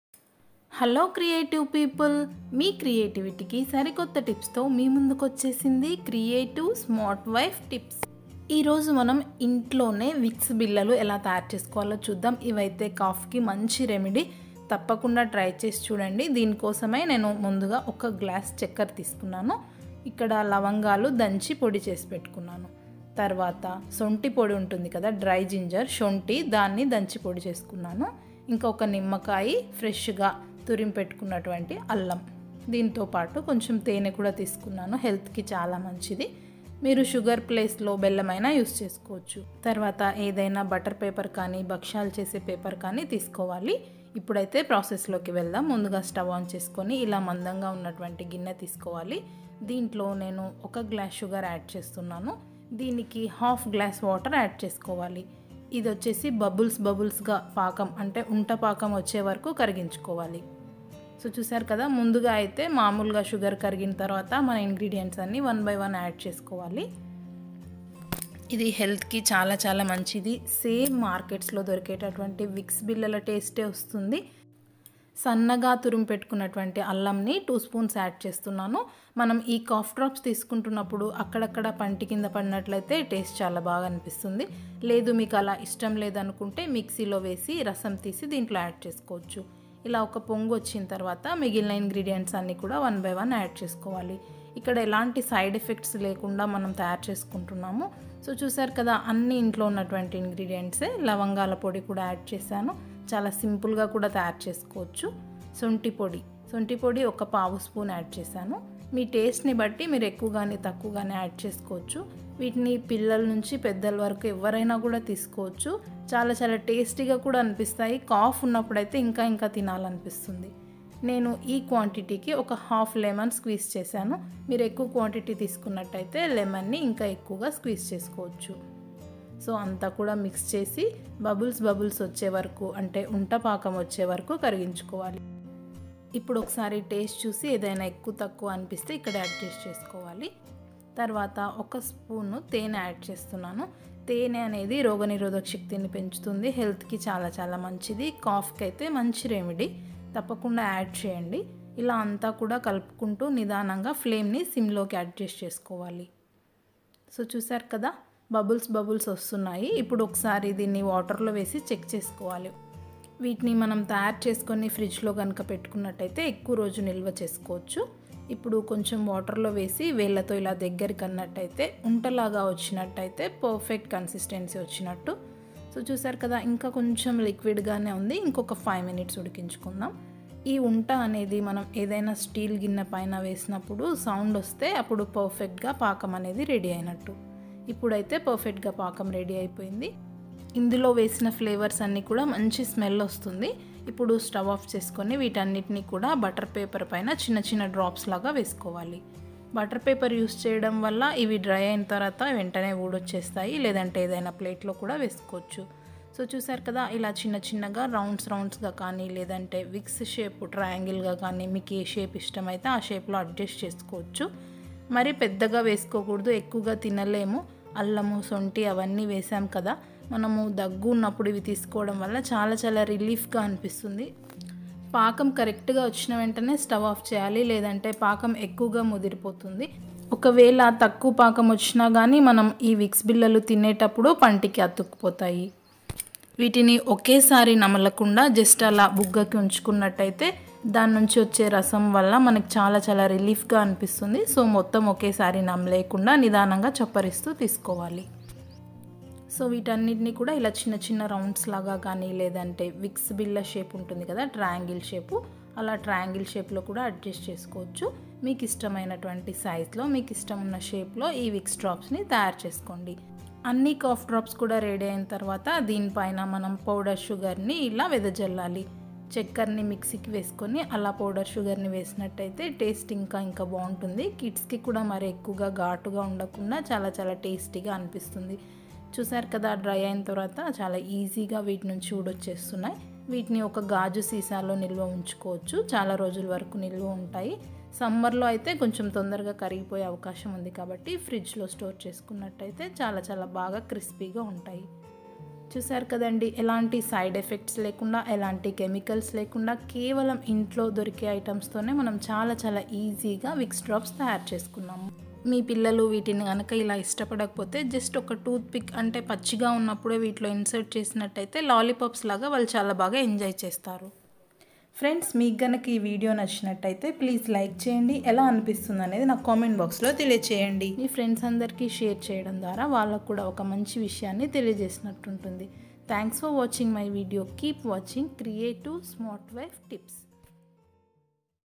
ఎంత భయంకరమైన దగ్గు కూడా మటుమాయం😯/Homemade cough syrup/cough home remedy in telugu/lockdown recipes
Here in this video i have prepared cough drops at home very easily and naturally, by using this cough drops u can feel very relief and no side effects because it is homemade.
i used lemon,ginger,cloves,sugar/jaggery,honey,dry ginger. take a butter paper and pour homemade cough syrup like small dumplings. sprinkle sugar powder on it. don't chew. just put it in mouth . very useful in cough time, must try in this lockdown. kids can also use

dry cough home remedy in telugu/tips for cough

simple home remedies,natural remedies, thank u for watching